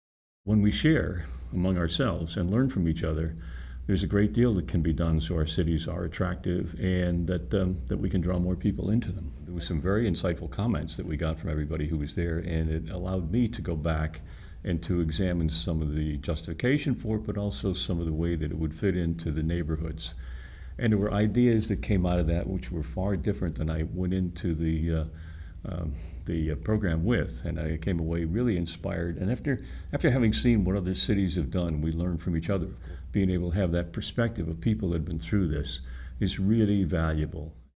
MIC: Reading, PA Mayor Tom McMahon
Reading (PA) Mayor Tom McMahon reflects on his experience with the Mayors' Institute on City Design.